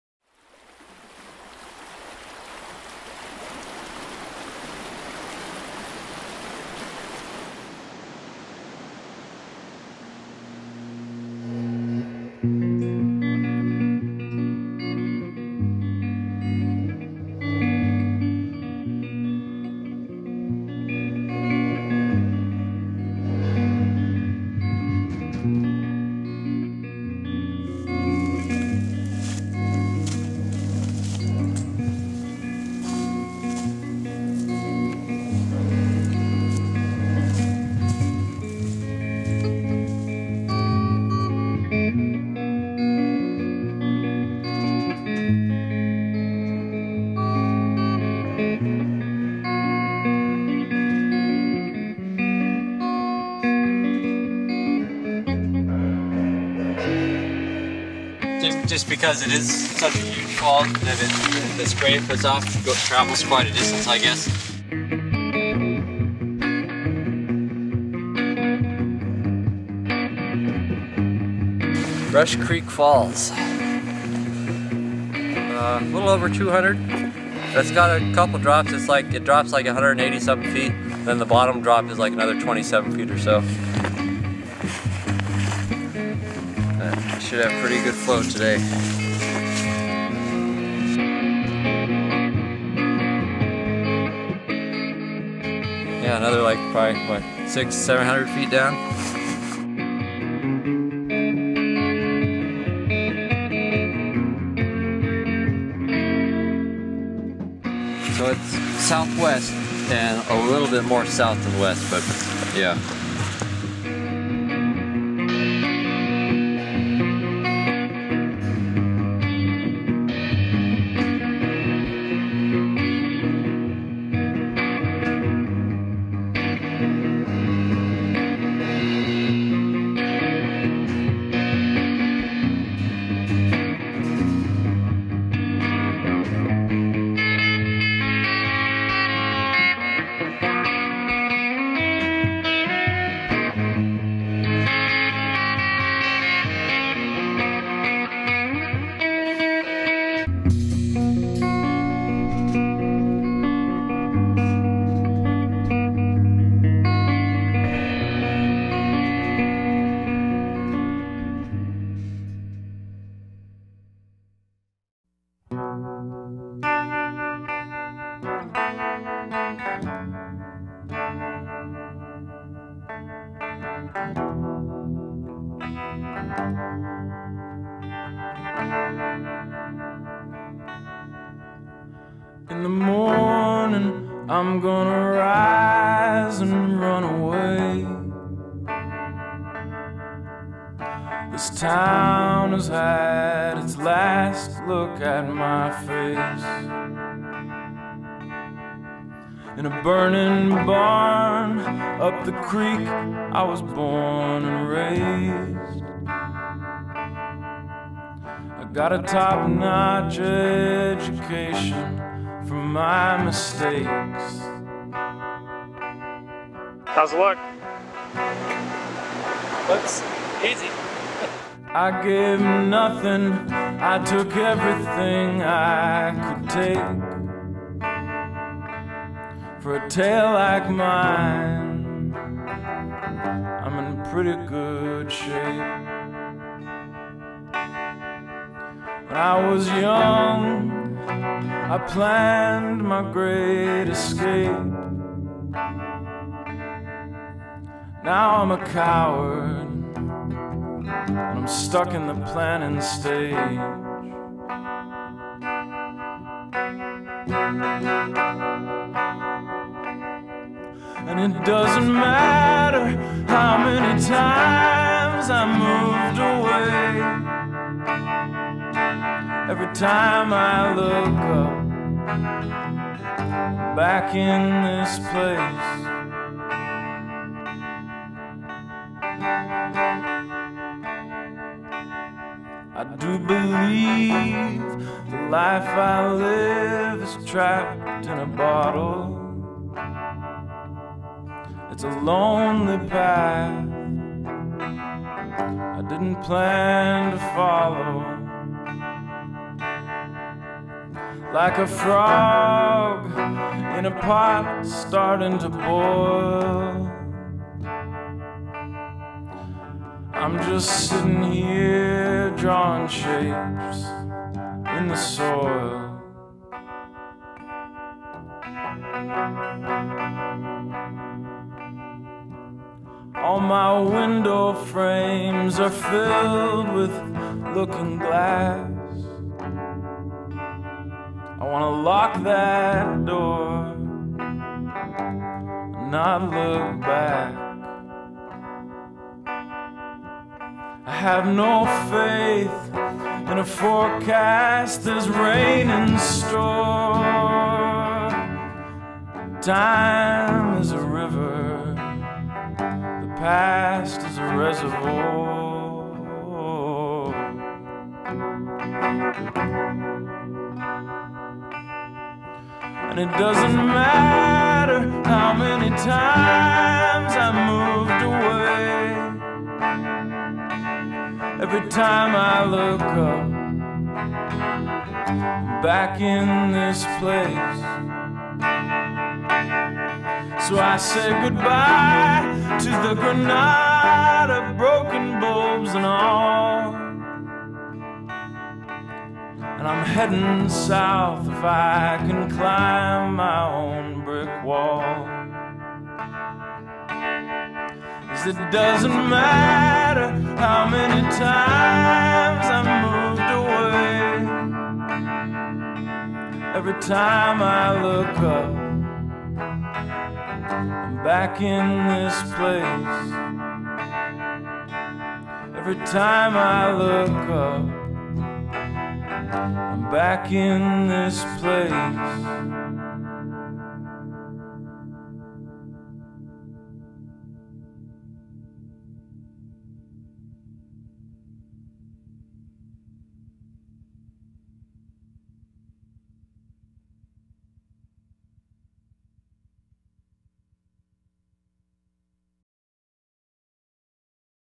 Joseph Broschart Photography. Remote Waterfalls Survey Vol.1
A documentary short looking at Joseph Broschart's process of accessing, surveying and photographing remote waterfalls of the Pacific Northwest.